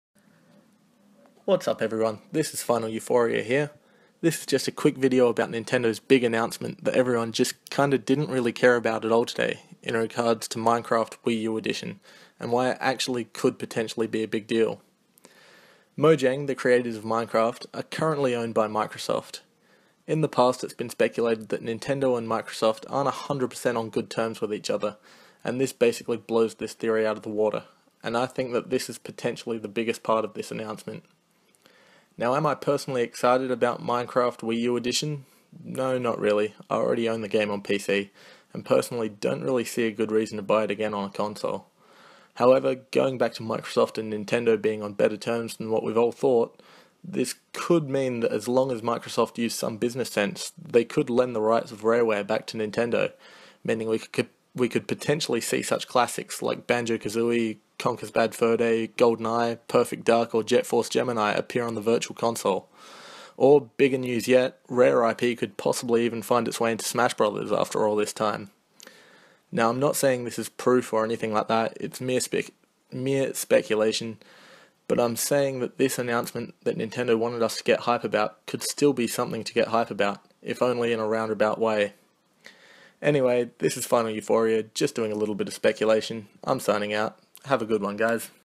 Nintendo's 7th December 2015 Announcement Flop... or is it?
Quick bit of speculation regarding todays 'meh' announcment from Nintendo in regards to Minecraft, Nintendo, Mojang, Microsoft, and a blast from the past?